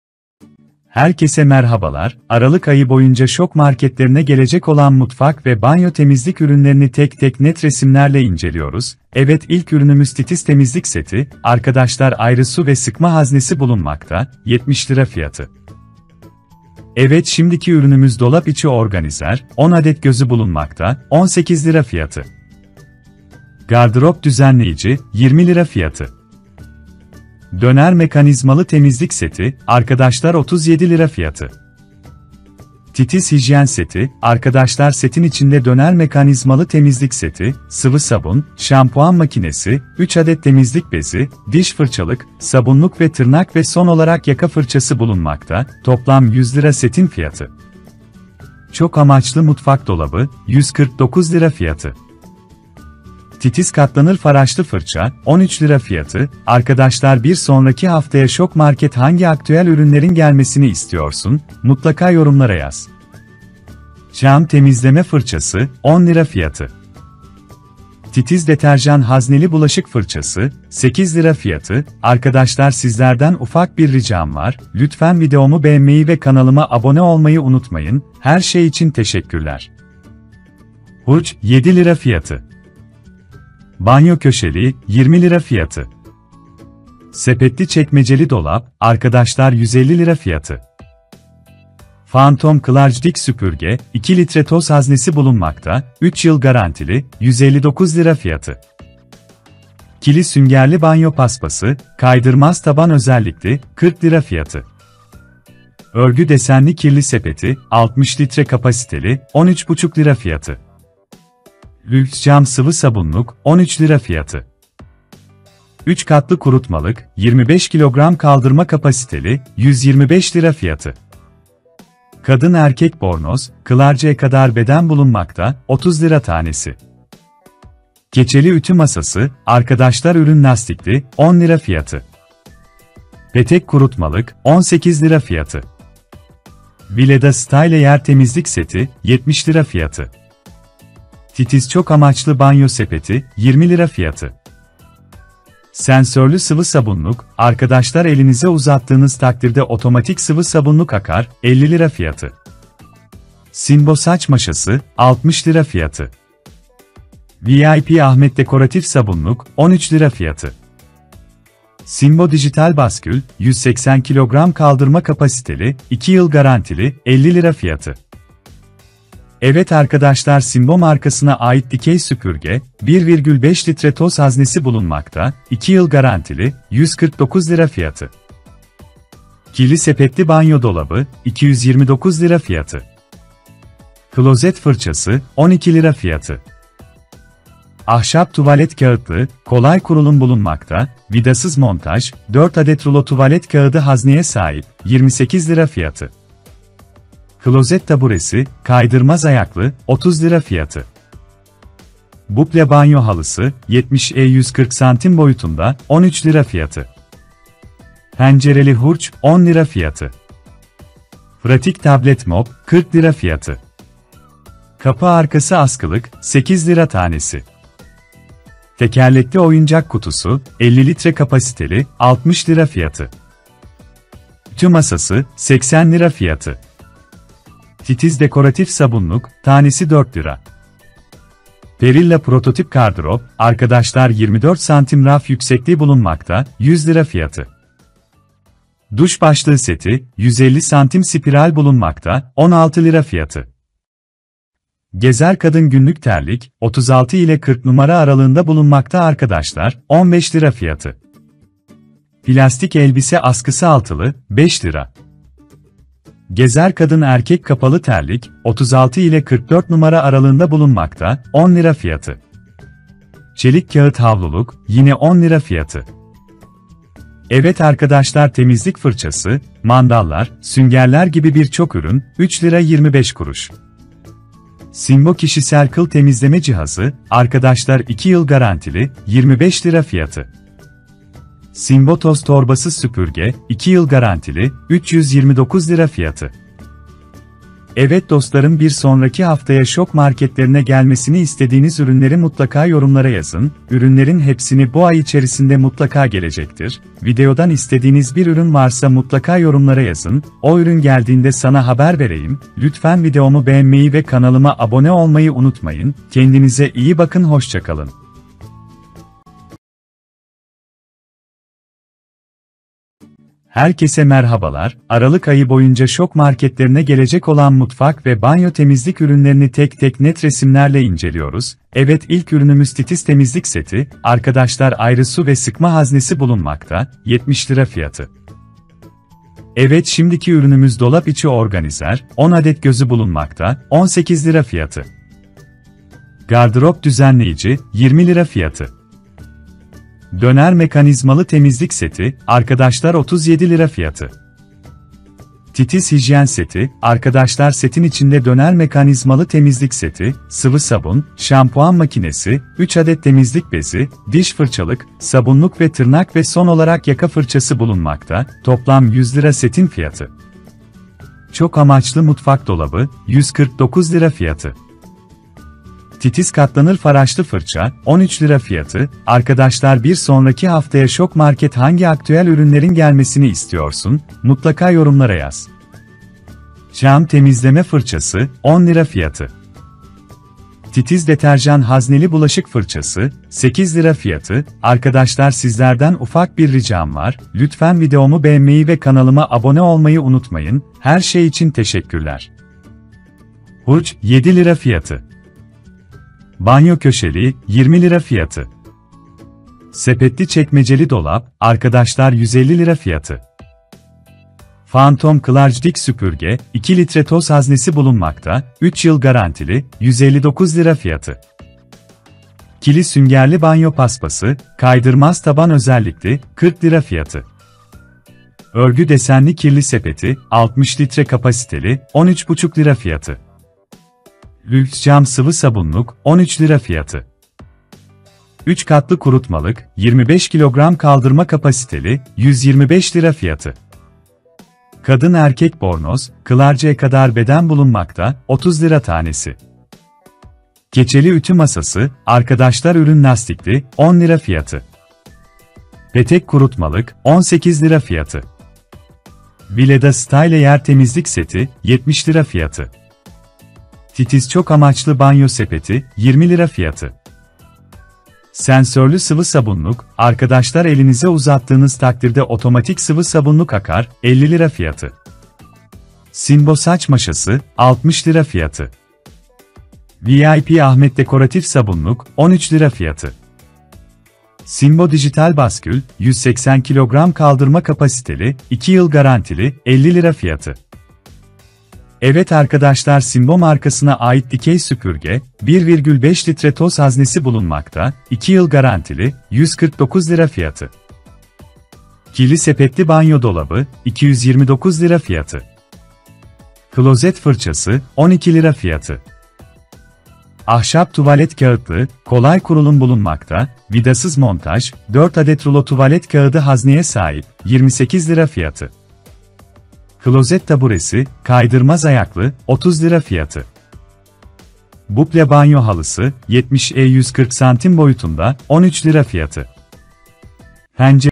Şok 15 Aralık 2020 Aktüel Ürünleri | Temizlik & Aksesuar & Banyo Mobilyası Ürünleri
Herkese merhabalar, aralık ayı boyunca şok marketlerine gelecek olan mutfak ve banyo temizlik ürünlerini tek tek net resimlerle inceliyoruz, evet ilk ürünümüz titiz temizlik seti, arkadaşlar ayrı su ve sıkma haznesi bulunmakta, 70 lira fiyatı.  evet şimdiki ürünümüz dolap içi organizer, 10 adet gözü bulunmakta, 18 lira fiyatı. gardrop düzenleyici, 20 lira fiyatı. döner mekanizmalı temizlik seti, arkadaşlar 37 lira fiyatı. titiz hijyen seti, arkadaşlar setin içinde döner mekanizmalı temizlik seti, sıvı sabun, şampuan makinesi, 3 adet temizlik bezi, diş fırçalık, sabunluk ve tırnak ve son olarak yaka fırçası bulunmakta, toplam 100 lira setin fiyatı. çok amaçlı mutfak dolabı, 149 lira fiyatı. titiz katlanır faraşlı fırça, 13 lira fiyatı, arkadaşlar bir sonraki haftaya Şok market hangi aktüel ürünlerin gelmesini istiyorsun, mutlaka yorumlara yaz. cam temizleme fırçası, 10 lira fiyatı. titiz deterjan hazneli bulaşık fırçası, 8 lira fiyatı, arkadaşlar sizlerden ufak bir ricam var, lütfen videomu beğenmeyi ve kanalıma abone olmayı unutmayın, her şey için teşekkürler. hurç, 7 lira fiyatı. banyo köşeliği, 20 lira fiyatı. sepetli çekmeceli dolap, arkadaşlar 150 lira fiyatı. Fantom xlarge dik süpürge, 2 litre toz haznesi bulunmakta, 3 yıl garantili, 159 lira fiyatı. ikili süngerli banyo paspası, kaydırmaz taban özellikli, 40 lira fiyatı. örgü desenli kirli sepeti, 60 litre kapasiteli, 13 buçuk lira fiyatı. lüks cam sıvı sabunluk, 13 lira fiyatı. 3 katlı kurutmalık, 25 kilogram kaldırma kapasiteli, 125 lira fiyatı. kadın erkek bornoz, xlarge e kadar beden bulunmakta, 30 lira tanesi. Keçeli ütü masası, arkadaşlar ürün lastikli, 10 lira fiyatı. Petek kurutmalık, 18 lira fiyatı. Vileda style yer temizlik seti, 70 lira fiyatı. titiz çok amaçlı banyo sepeti, 20 lira fiyatı. sensörlü sıvı sabunluk, arkadaşlar elinize uzattığınız  takdirde otomatik sıvı sabunluk Akar, 50 lira fiyatı. Sinbo saç maşası, 60 lira fiyatı. VIP Ahmet dekoratif sabunluk, 13 lira fiyatı. Sinbo dijital baskül, 180 kilogram kaldırma kapasiteli, 2 yıl garantili, 50 lira fiyatı. evet arkadaşlar Sinbo markasına ait dikey süpürge, 1,5 litre toz haznesi bulunmakta, 2 yıl garantili, 149 lira fiyatı. kirli sepetli banyo dolabı, 229 lira fiyatı. klozet fırçası, 12 lira fiyatı. ahşap tuvalet kağıtlığı, kolay kurulum bulunmakta, vidasız montaj, 4 adet rulo tuvalet kağıdı hazneye sahip, 28 lira fiyatı. klozet taburesi, kaydırmaz ayaklı, 30 lira fiyatı. bukle banyo halısı, 70'e 140 santim boyutunda, 13 lira fiyatı. pencereli hurç, 10 lira fiyatı. pratik tablet mop, 40 lira fiyatı. kapı arkası askılık, 8 lira tanesi. tekerlekli oyuncak kutusu, 50 litre kapasiteli, 60 lira fiyatı. ütü masası, 80 lira fiyatı. titiz dekoratif sabunluk, tanesi 4 lira. perilla prototip gardırop, arkadaşlar 24 santim raf yüksekliği bulunmakta, 100 lira fiyatı. duş başlığı seti, 150 santim spiral bulunmakta, 16 lira fiyatı. Gezer kadın günlük terlik, 36 ile 40 numara aralığında bulunmakta arkadaşlar, 15 lira fiyatı. plastik elbise askısı altılı, 5 lira. Gezer kadın erkek kapalı terlik, 36 ile 44 numara aralığında bulunmakta, 10 lira fiyatı. Çelik kağıt havluluk, yine 10 lira fiyatı. evet arkadaşlar temizlik fırçası, mandallar, süngerler gibi bir çok ürün, 3 lira 25 kuruş. Sinbo kişisel kıl temizleme cihazı, arkadaşlar 2 yıl garantili, 25 lira fiyatı. Sinbo toz torbasız süpürge, 2 yıl garantili, 329 lira fiyatı. evet dostlarım Bir sonraki haftaya şok marketlerine gelmesini istediğiniz ürünleri mutlaka yorumlara yazın, ürünlerin hepsini bu ay içerisinde mutlaka gelecektir,  videodan istediğiniz bir ürün varsa mutlaka yorumlara yazın, o ürün geldiğinde sana haber vereyim, lütfen videomu beğenmeyi ve  kanalıma abone olmayı unutmayın, kendinize iyi bakın hoşçakalın